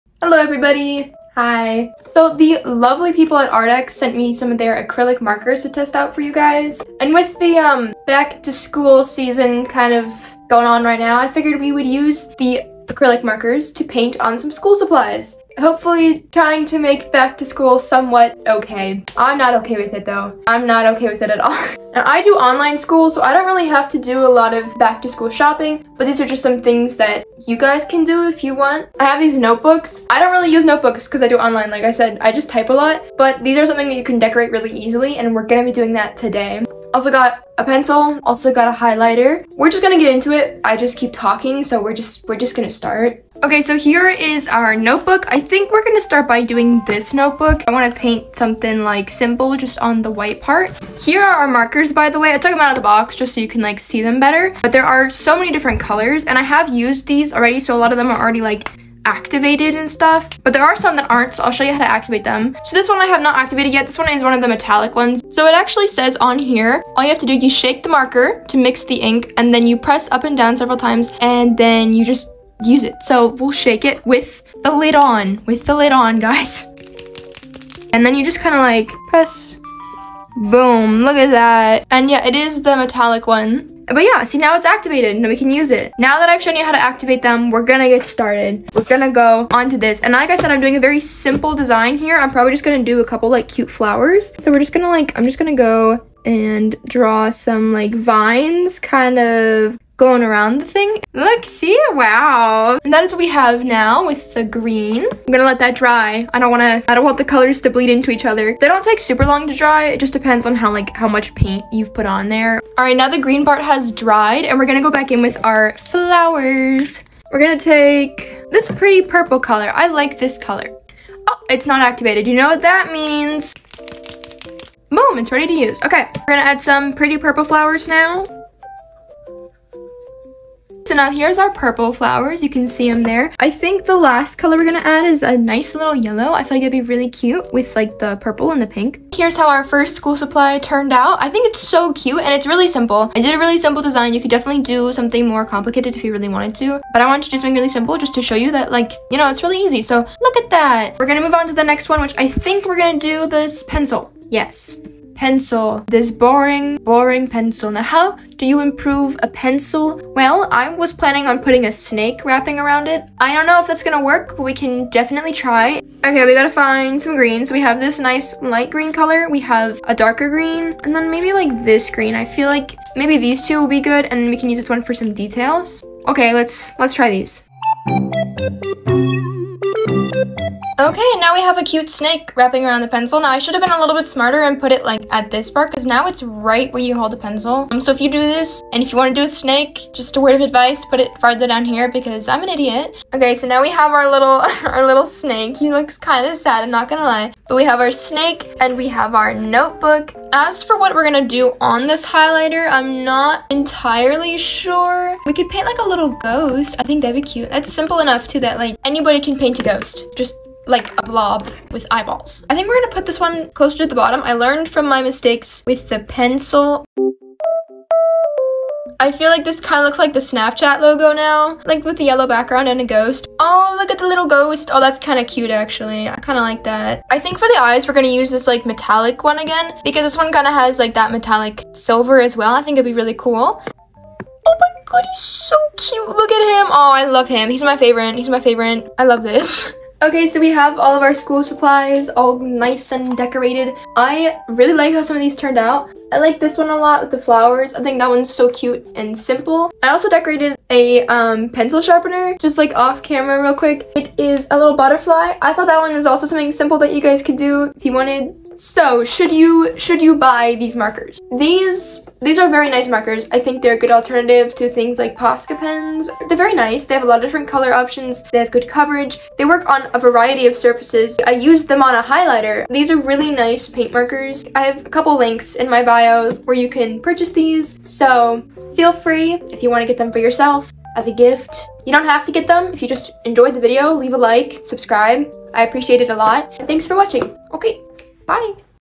Painting on school supplies (ft. Arrtx acrylic markers)
Hope everyone’s having a fun back to school season 😭😭! Art always makes everything better… which is why in this video i paint on school supplies :)

Links to purchase the markers!👇

Arrtx 48 acrylic markers,15%off, code: S4POUTNY (August 10th~15th 23:59PDT)

Arrtx 36 acrylic markers,15%off,code: S4POUTNY (August 10th~15th 23:59PDT)